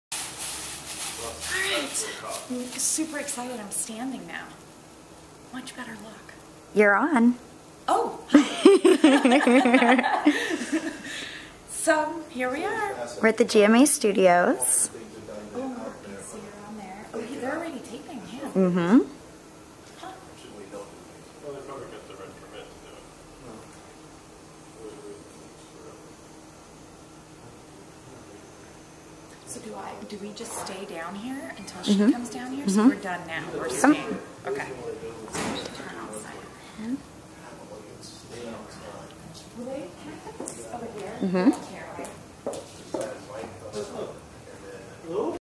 Behind the scenes at Good Morning America Health in NYC!
Behind the scenes at Good Morning America showing the world Beautisol self-tanner.